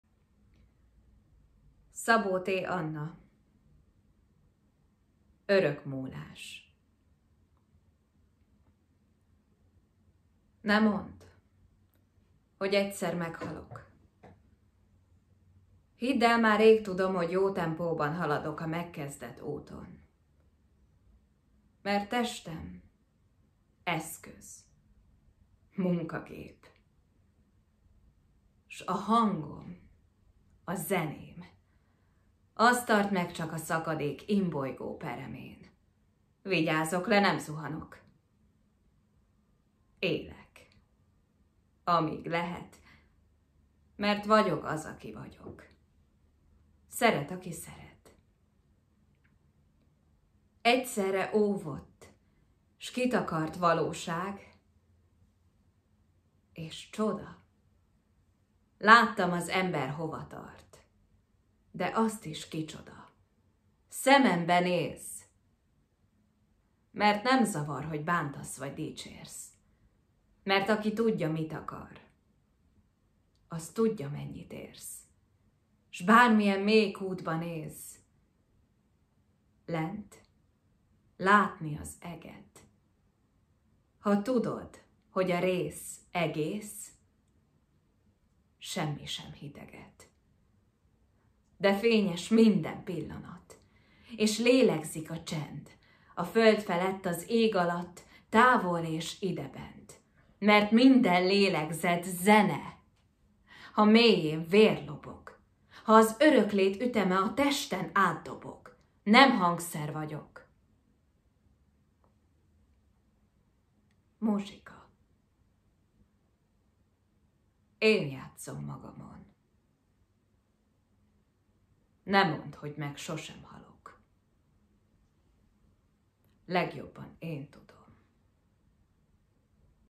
Szabó Gréta versmondás: Szabó T. Anna - Örökmúlás
Szabó Gréta, a Muharay Színház művésze múlt héten egy blogbejegyzésben mutatta be nekünk Szabó T. Annát:

Most pedig egy versmondást hozott tőle Örökmúlás címmel.

2020. március 23-án elindult az online Vörösmarty Művelődési Ház, melynek keretében a nálunk lévő csoportvezetők, művészek készítenek vlog, illetve blog bejegyzéseket, ezzel segítve, hogy működésünket a virtuális térben folytatni tudjuk.